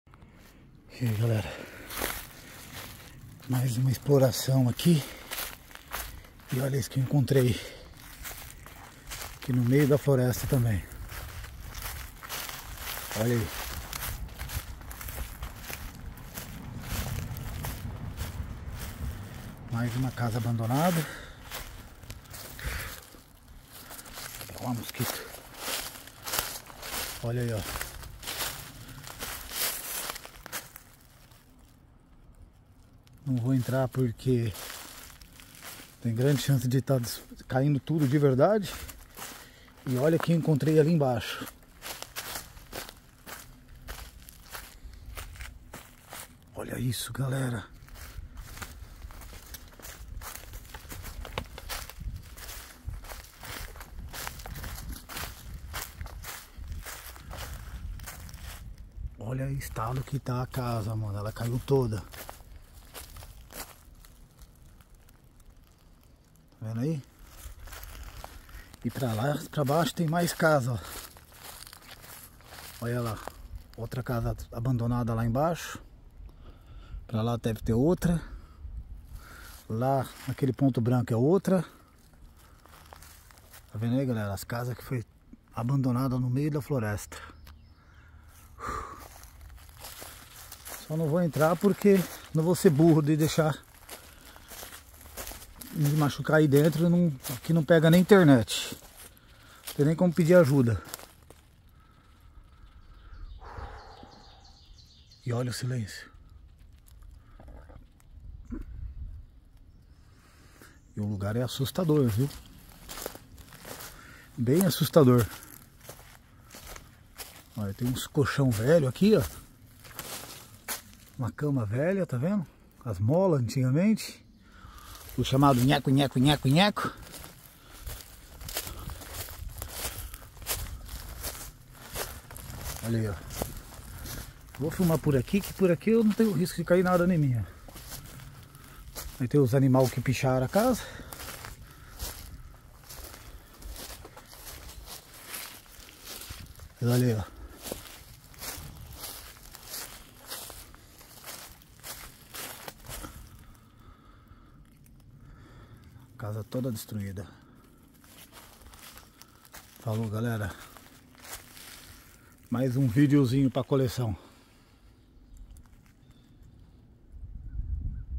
Medo tomou conta
Outra vila no meio da floresta ,lugar muito estranho .